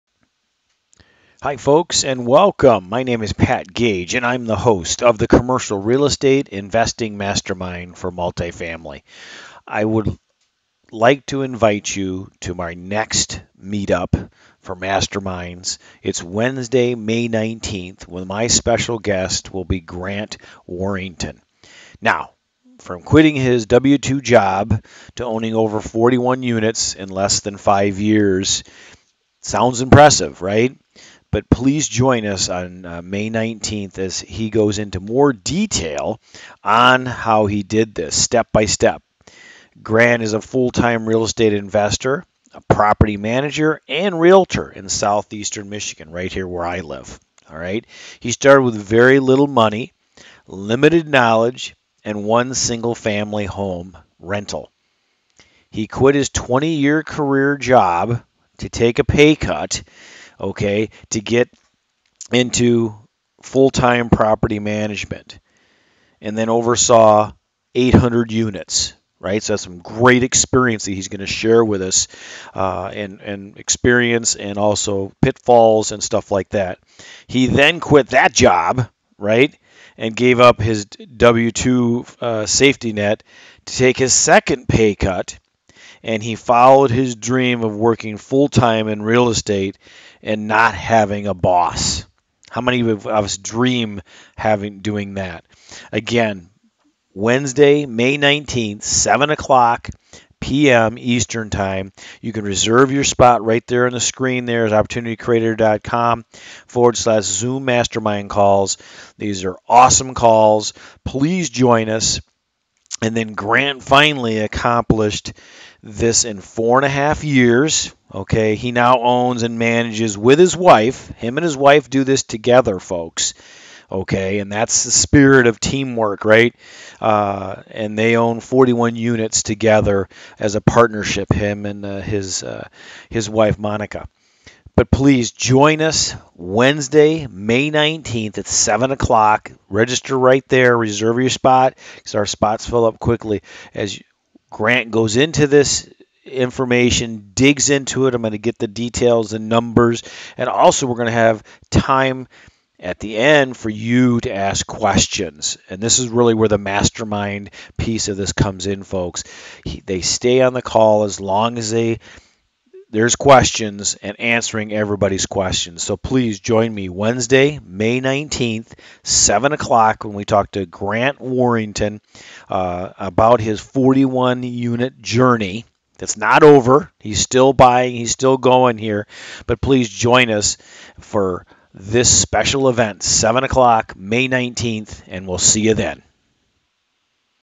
Meetup Speaker May 19 2021 Grant Warrington in Less 5 years has 41 unit portfolio- Here his Story
The May 19th. Mastermind with Grant Warrington of Warrington Capital.

In this video Pat Gage talks with Grant Warrington who is a is full time Real Estate Investor, Property Manager & Realtor in Southeast Michigan.  He started with very little money, limited knowledge and 1 single family rental.

He quit his 20 year W2 job to take a pay cut and get into full time property management and oversaw around 800 units locally.  He then quit the property management job and gave up the W2 safety net to take his second pay cut and he followed his dream of working full time in Real Estate and not having a boss.

He accomplished this in approximily 4 ½ years. He now owns and manages along with his wife Monika their 41 unit portfolio.

Come and hear his story about the ups and downs of his real estate career on Wednesday May 19th. at 7 PM EDT. Bring your questions and an open mind!

When: May 19, 2021
Time: 7:00 PM Eastern Time (US and Canada)